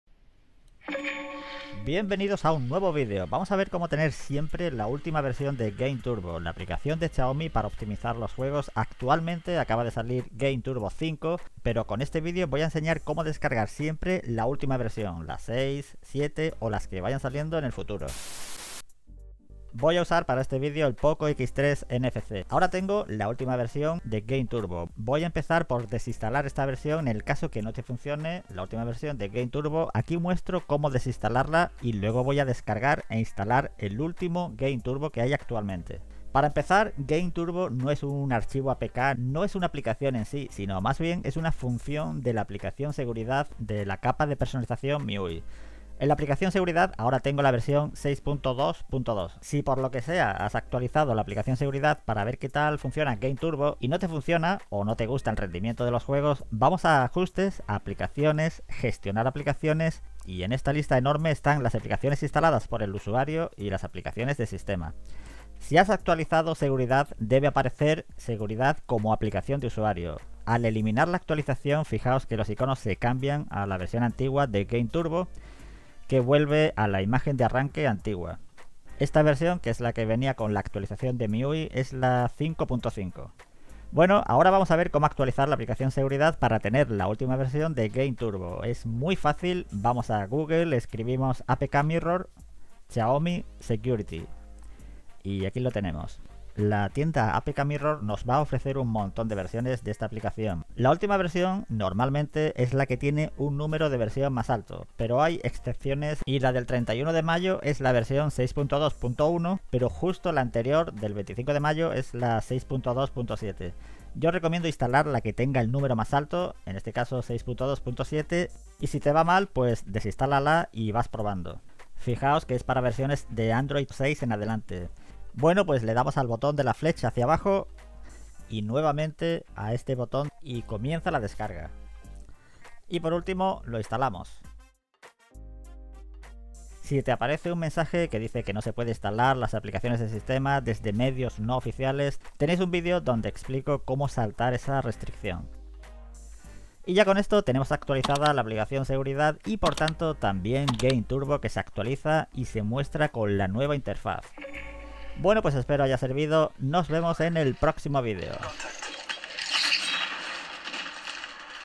Como tener Game Turbo de MIUI SIEMPRE Acualizado a la Última Versión
Como instalar actualizaciones de Game Turbo de MIUI a la última versión y tambien como desinstalar las actualizaciones en el caso que no te funcione correctamente.